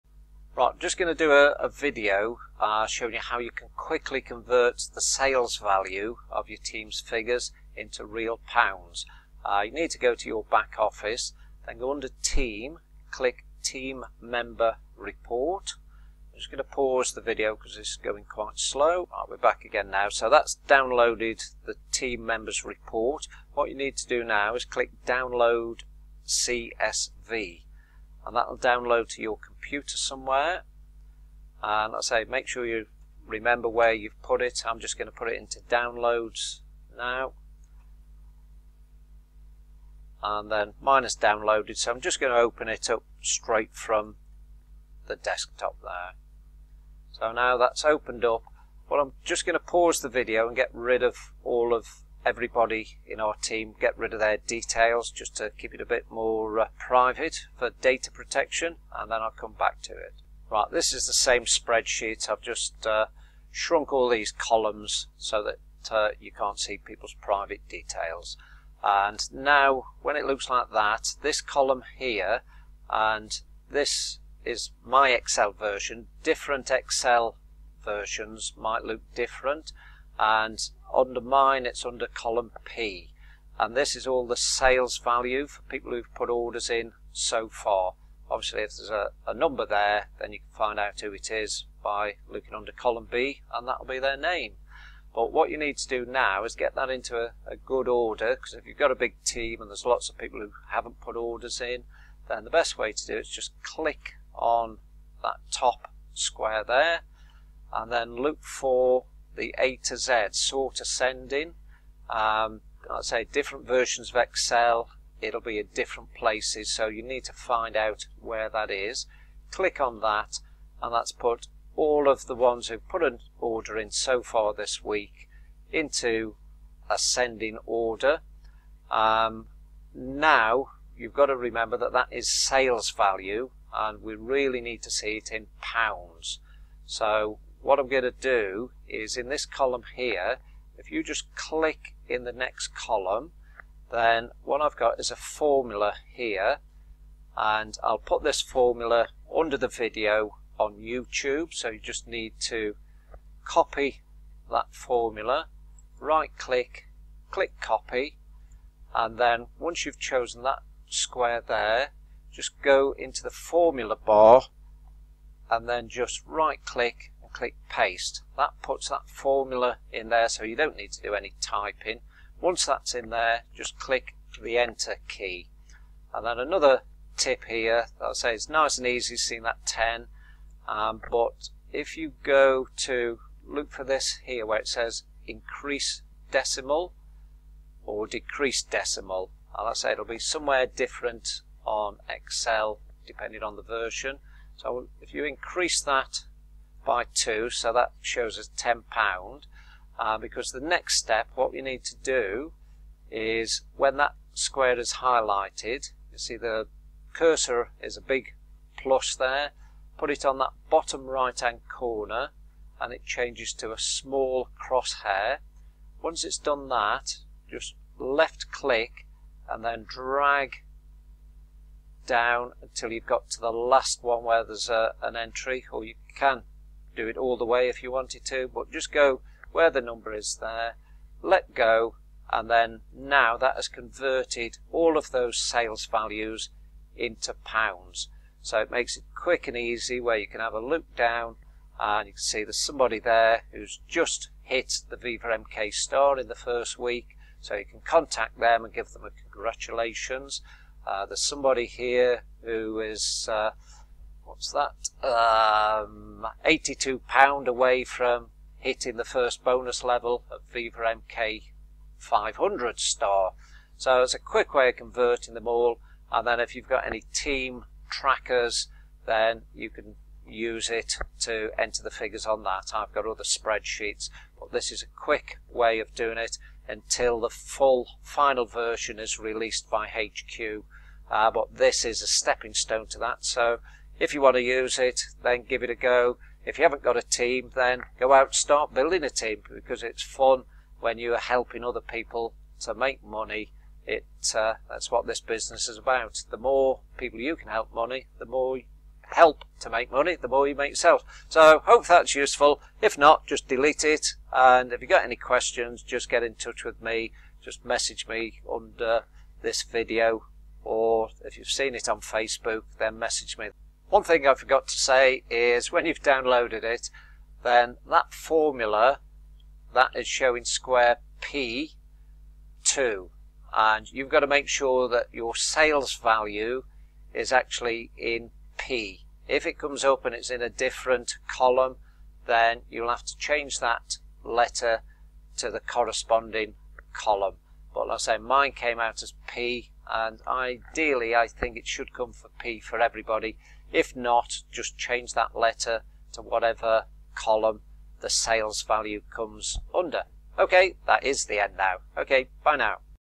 How to quickly convert all of your team figures from Sales Value to £s
How to quickly convert all of your team figures from Sales Value £s to REAL £s - this will be your true Retail Orders with NO Sales aids. these are the figures you need to know for reaching your Bonus levels.

This is the formula you need to copy & paste into the spreadsheet - =((P2*1.2)/75)*100

PLEASE ensure that the column that your Sales Value figures are under is 'P' - if not, make sure you change the P in the formula to the correct letter for the sales Value letter

kl891663